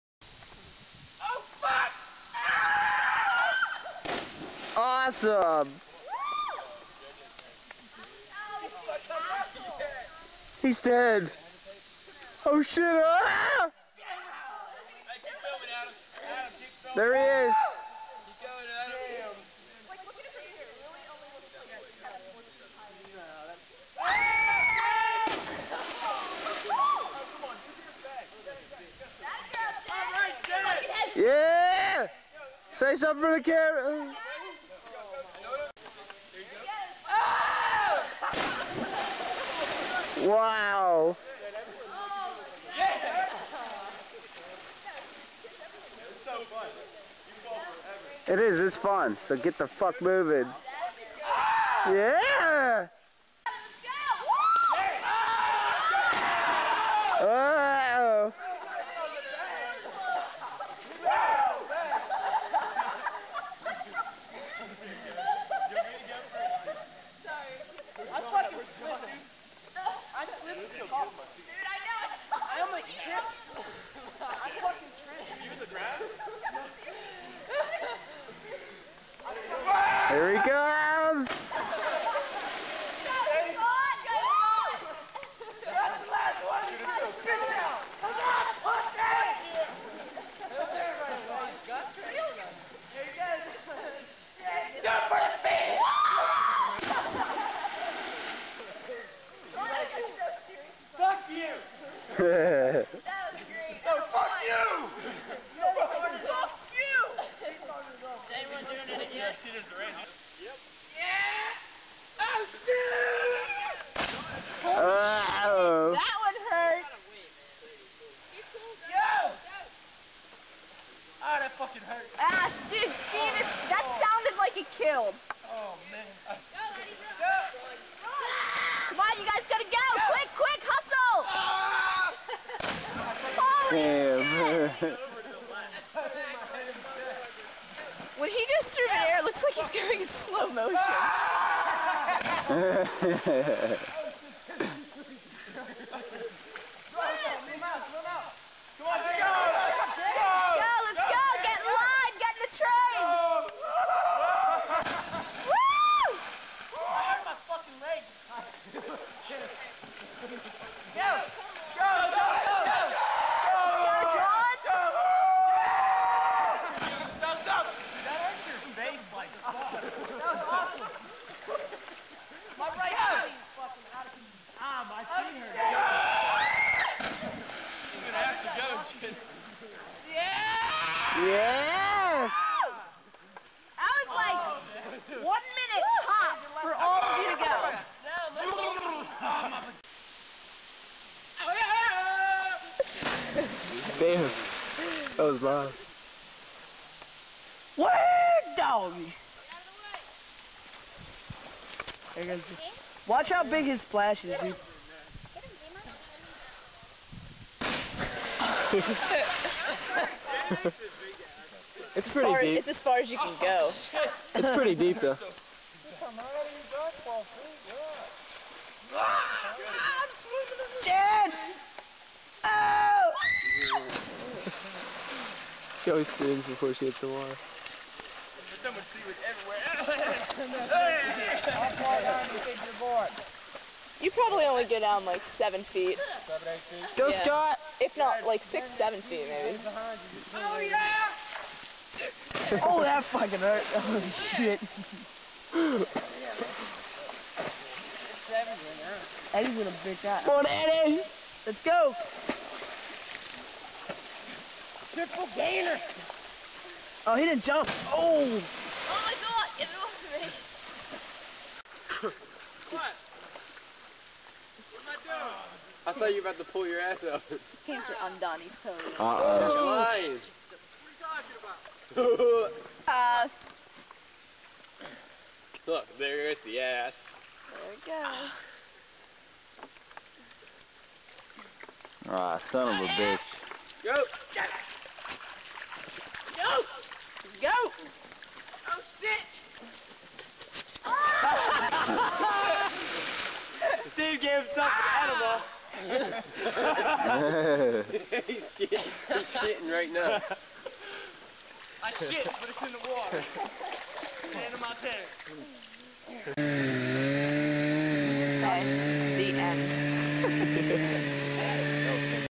Jumping off Cliffs
a bunch of us jumping off cliffs up at Jon's farm